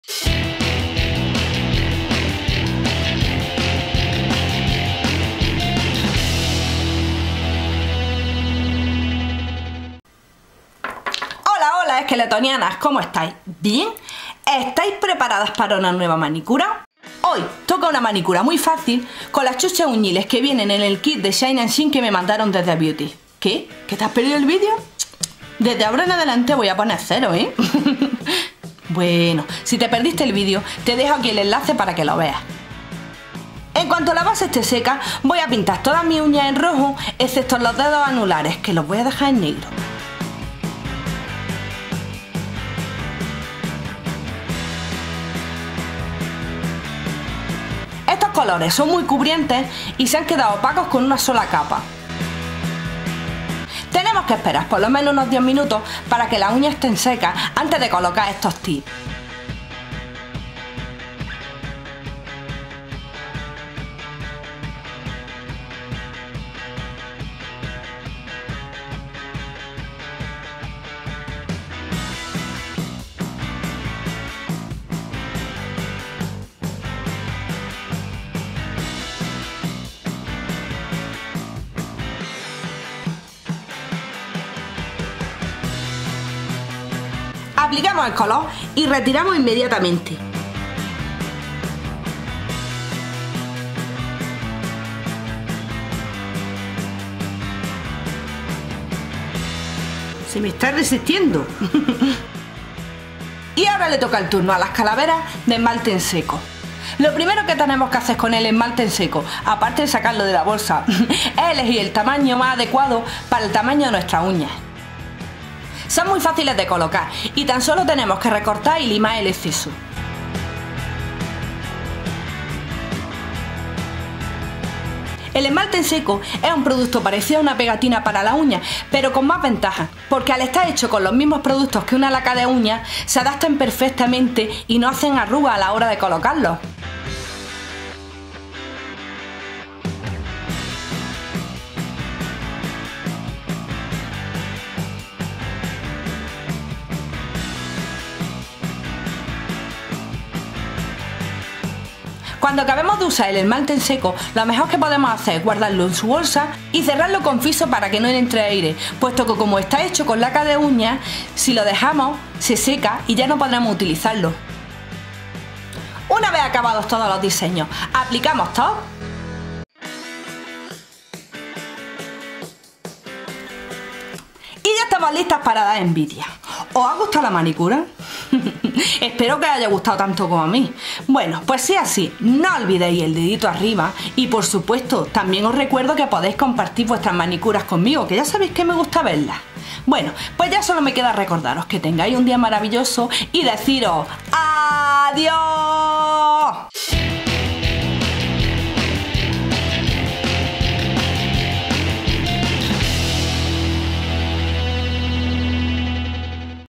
Rockea tus uñas con shine and sheen
HolaHola!! Elige los colores que vayan con tu look y disfruta!
HelloHello!! Choose the color of your look and enjoy!

CREA ANILLOS CON LACA DE UÑAS/ Do your rings with nailpolish

Espero que os guste!!! no olvideis dejar vuestros comentarios y el dedito arriba!! Muacck